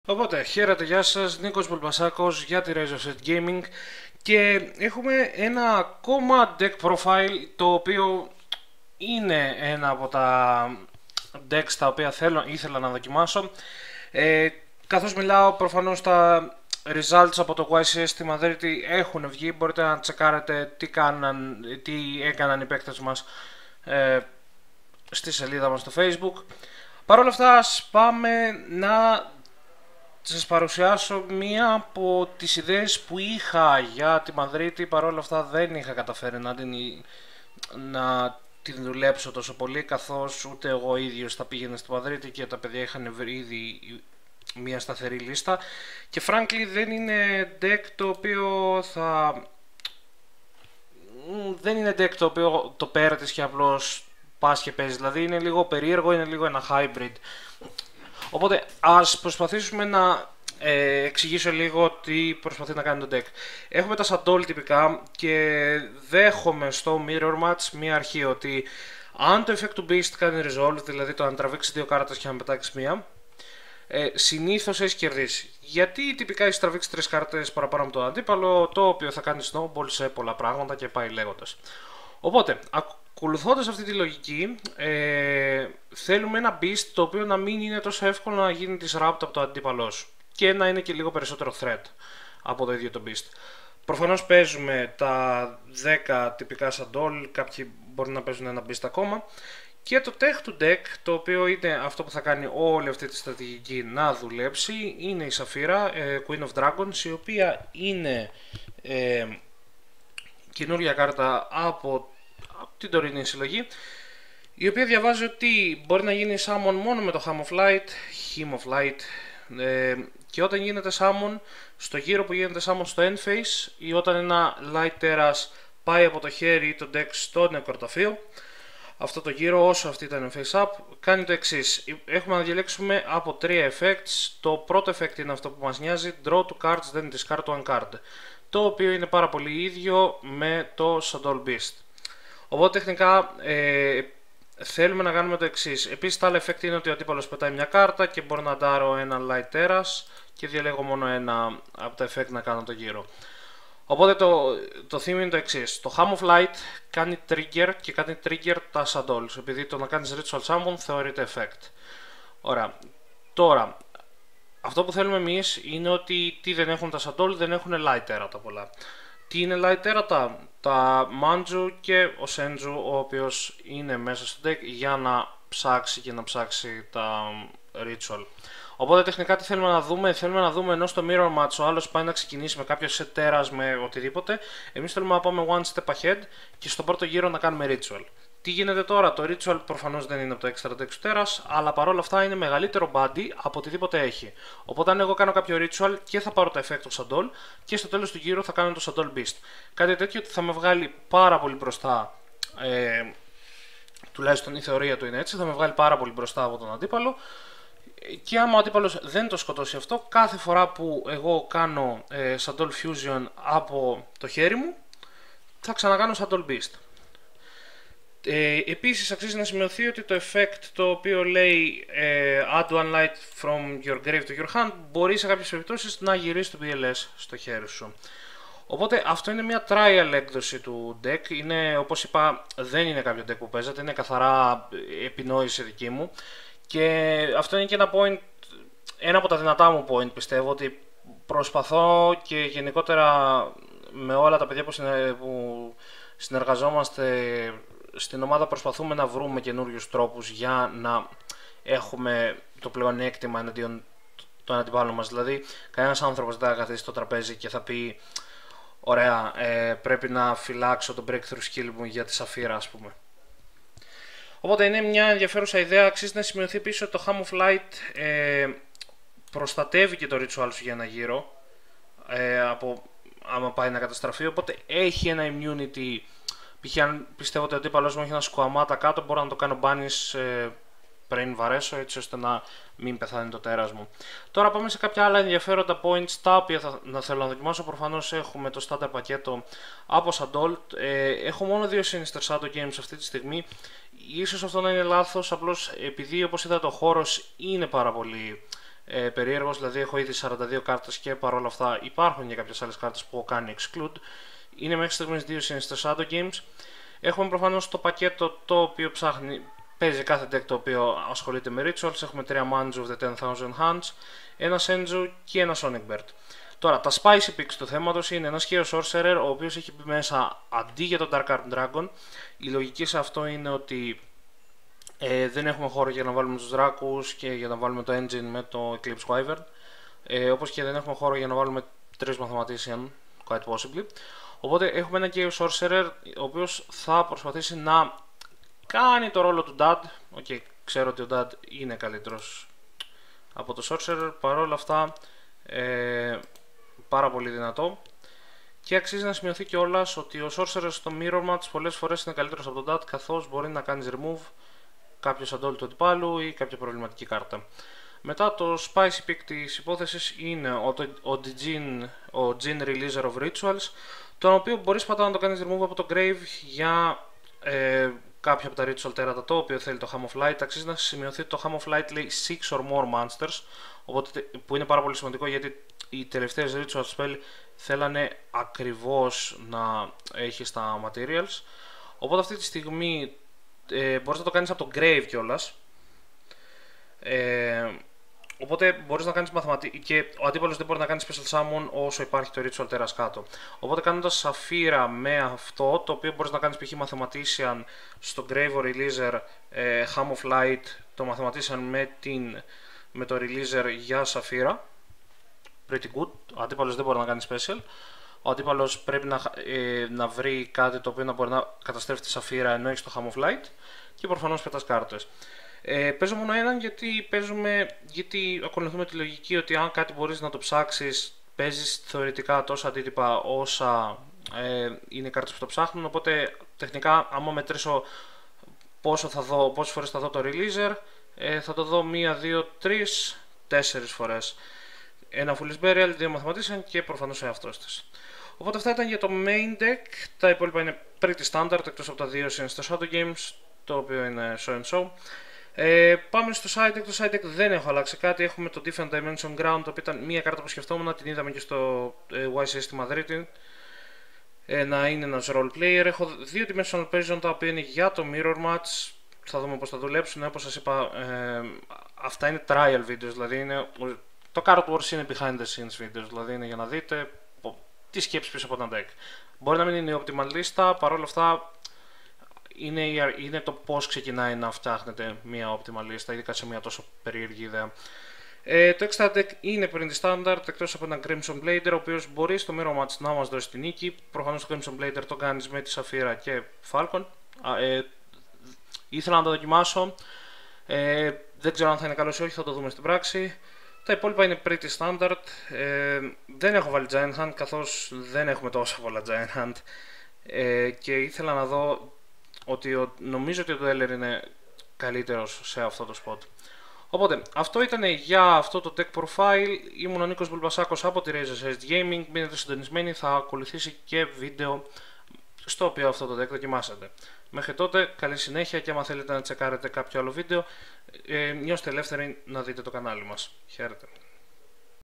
Yu-Gi-Oh! Deckprofile Saffira Shaddoll Sept 2014
Yu-Gi-Oh! Deck Profile for Saffira Shaddolls by YGO.teamREG,Nick Boulbasakos